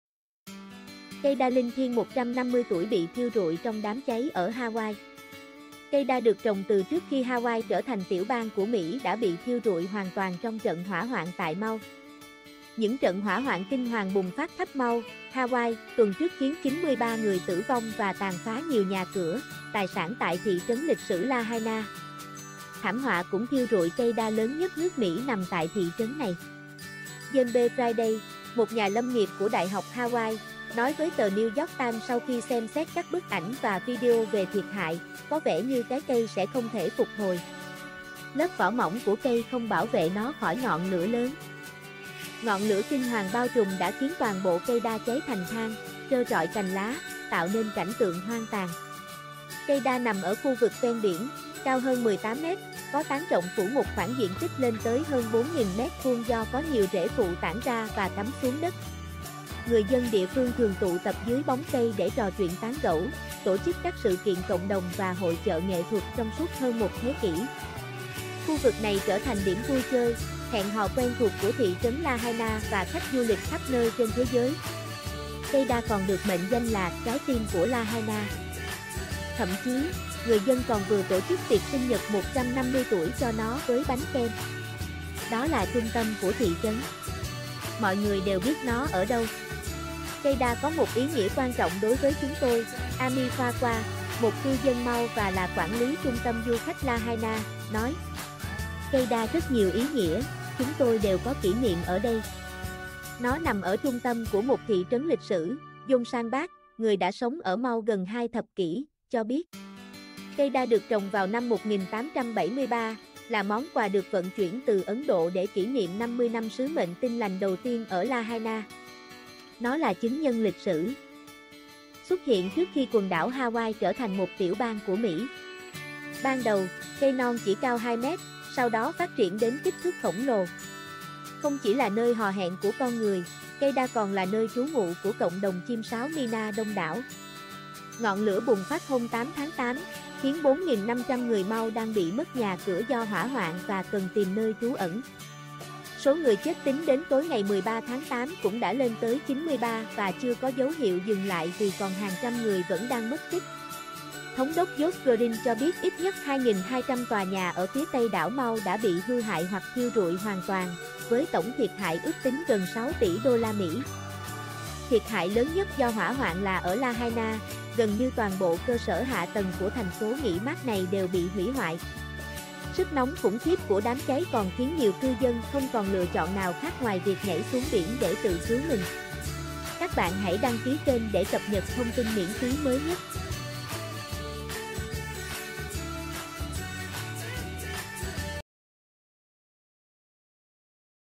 Cây đa linh thiêng 150 tuổi bị thiêu rụi trong đám cháy ở Hawaii
Cây đa được trồng từ trước khi Hawaii trở thành tiểu bang của Mỹ đã bị thiêu rụi hoàn toàn trong trận hỏa hoạn tại Maui.

Những trận hỏa hoạn kinh hoàng bùng phát khắp Maui (Hawaii) tuần trước khiến 93 người tử vong và tàn phá nhiều nhà cửa, tài sản tại thị trấn lịch sử Lahaina. Thảm họa cũng thiêu rụi cây đa lớn nhất nước Mỹ nằm tại thị trấn này.

James B Friday, một nhà lâm nghiệp của Đại học Hawaii, nói với tờ New York Times sau khi xem xét các bức ảnh và video về thiệt hại: "Có vẻ như cái cây sẽ không thể phục hồi. Lớp vỏ mỏng của cây không bảo vệ nó khỏi ngọn lửa lớn". Ngọn lửa kinh hoàng bao trùm đã khiến toàn bộ cây đa cháy thành than, trơ trọi cành lá, tạo nên cảnh tượng hoang tàn.

Cây đa nằm ở khu vực ven biển, cao hơn 18 mét, có tán rộng phủ một khoảng diện tích lên tới hơn 4.000 mét vuông do có nhiều rễ phụ tản ra và cắm xuống đất. Người dân địa phương thường tụ tập dưới bóng cây để trò chuyện tán gẫu, tổ chức các sự kiện cộng đồng và hội chợ nghệ thuật trong suốt hơn một thế kỷ. Khu vực này trở thành điểm vui chơi, hẹn hò quen thuộc của thị trấn Lahaina và khách du lịch khắp nơi trên thế giới. Cây đa còn được mệnh danh là "trái tim của Lahaina", thậm chí, người dân còn vừa tổ chức tiệc sinh nhật 150 tuổi cho nó với bánh kem.

"Đó là trung tâm của thị trấn. Mọi người đều biết nó ở đâu. Cây đa có một ý nghĩa quan trọng đối với chúng tôi", Amy Fuqua, một cư dân Maui và là quản lý trung tâm du khách Lahaina, nói.

"Cây đa rất nhiều ý nghĩa, chúng tôi đều có kỷ niệm ở đây. Nó nằm ở trung tâm của một thị trấn lịch sử", John Sandbach, người đã sống ở Maui gần hai thập kỷ, cho biết.

Cây đa được trồng vào năm 1873, là món quà được vận chuyển từ Ấn Độ để kỷ niệm 50 năm sứ mệnh Tin lành đầu tiên ở Lahaina. Nó là chứng nhân lịch sử, xuất hiện trước khi quần đảo Hawaii trở thành một tiểu bang của Mỹ. Ban đầu, cây non chỉ cao hai mét, sau đó phát triển đến kích thước khổng lồ. Không chỉ là nơi hò hẹn của con người, cây đa còn là nơi trú ngụ của cộng đồng chim sáo myna đông đảo.

Ngọn lửa bùng phát hôm 8/8, khiến 4.500 người Maui đang bị mất nhà cửa do hỏa hoạn và cần tìm nơi trú ẩn. Số người chết tính đến tối 13/8 cũng đã lên tới 93 và chưa có dấu hiệu dừng lại vì còn hàng trăm người vẫn đang mất tích. Thống đốc Josh Green cho biết ít nhất 2.200 tòa nhà ở phía tây đảo Maui đã bị hư hại hoặc thiêu rụi hoàn toàn, với tổng thiệt hại ước tính gần 6 tỷ USD. Thiệt hại lớn nhất do hỏa hoạn là ở Lahaina, gần như toàn bộ cơ sở hạ tầng của thành phố nghỉ mát này đều bị hủy hoại. Sức nóng khủng khiếp của đám cháy còn khiến nhiều cư dân không còn lựa chọn nào khác ngoài việc nhảy xuống biển để tự cứu mình.